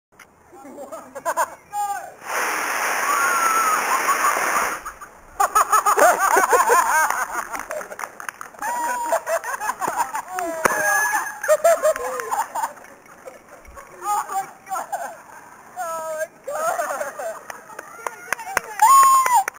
Rocket powered skates
Dogsbody straps a pair of rockets to his skates!